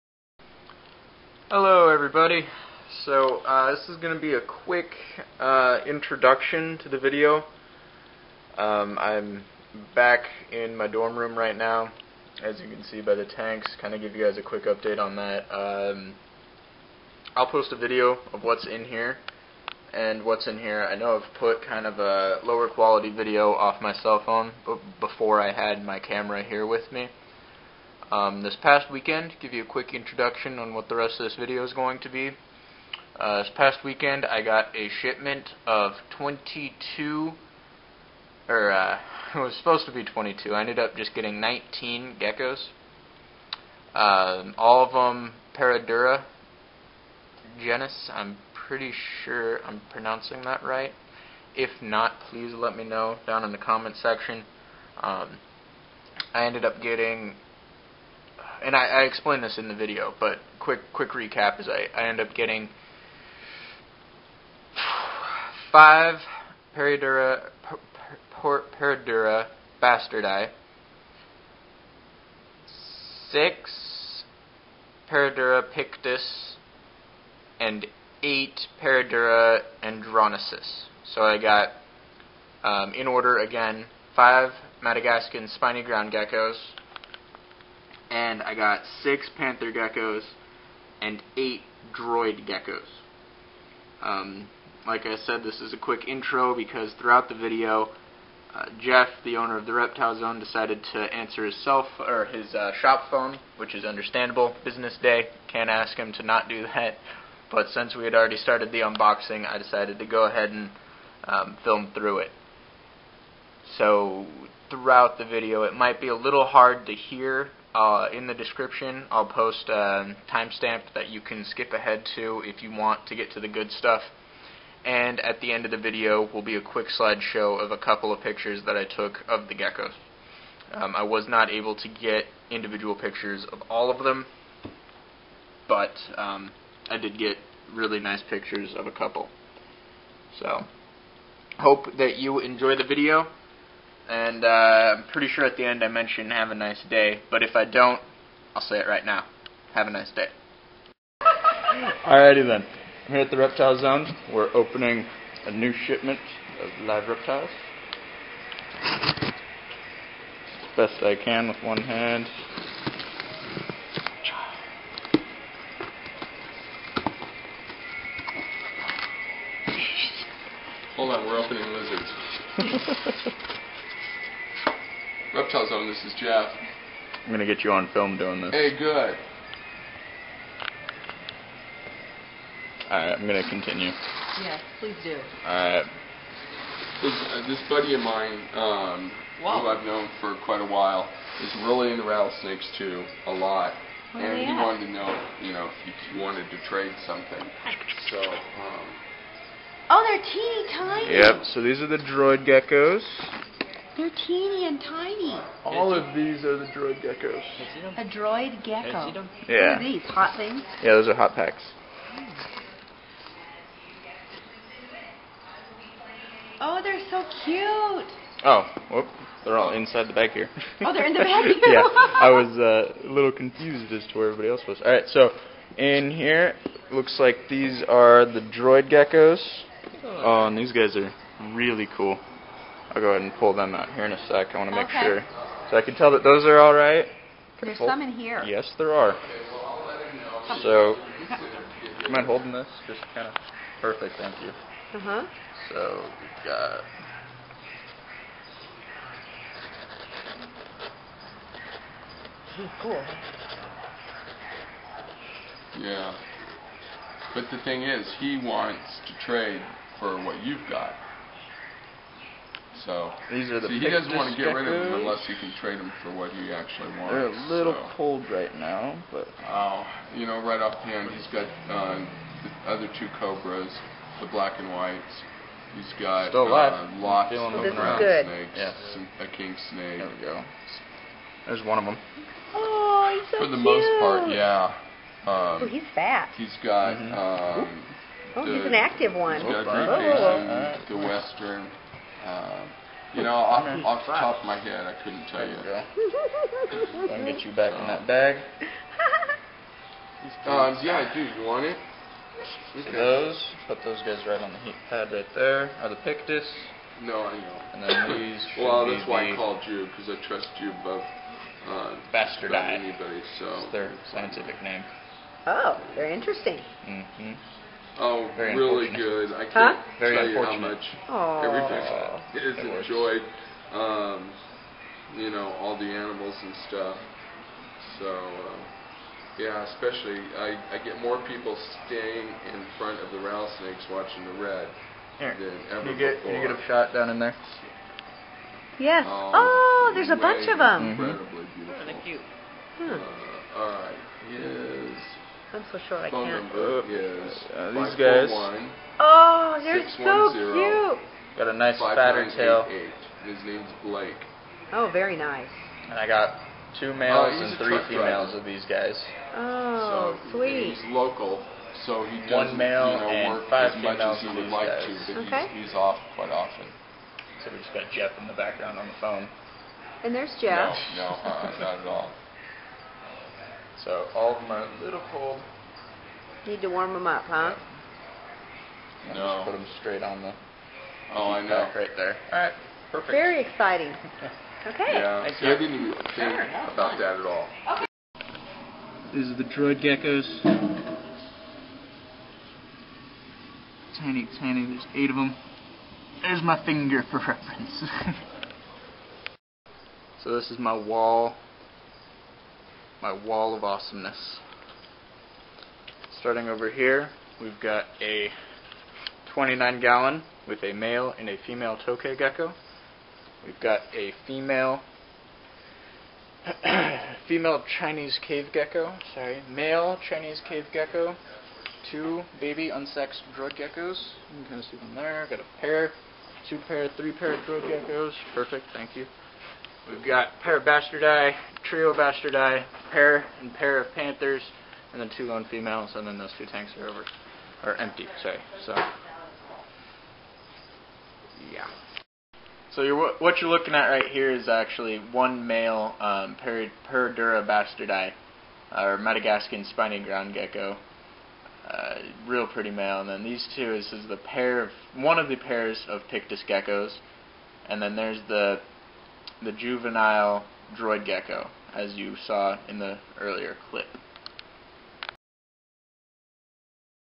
19 paroedura gecko unboxing!
Watch as I unbox 19 geckos from the genus Paroedura.
2.3 bastardi, or spiny ground gecko
2.4 pictus, or panther gecko
3.3.2 androyensis, or droid gecko

unboxing at.